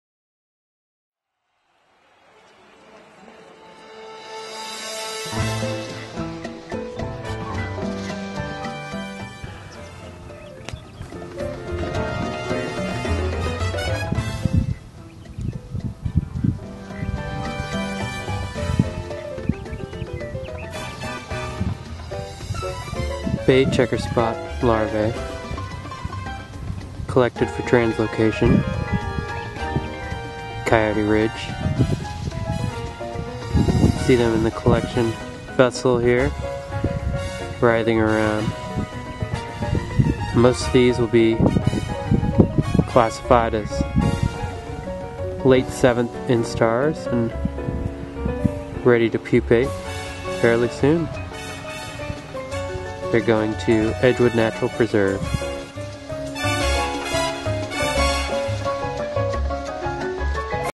Bay Checkerspot Larvae in action
A short video of collected larvae moving around in cooled yogurt
container. February 2013. Coyoyte Ridge.